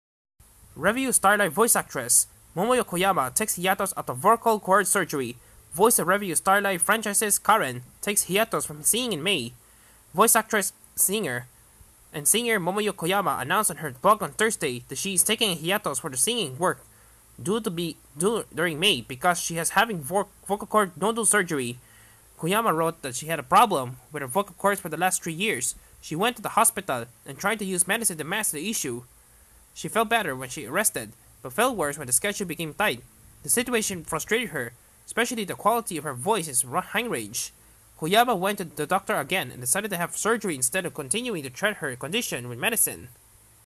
Revue Starlight Voice Actress Momoyo Koyama Takes Hiatus After Vocal Cord Surgery
Voice of Revue Starlight franchise's Karen takes hiatus from singing in May